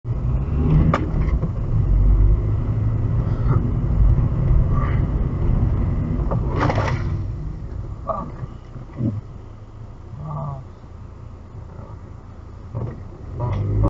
Dashcam West Yorkshire Huddersfield Westbourne Road
I must be invisible, Driver pulls out in front of me i braked just in time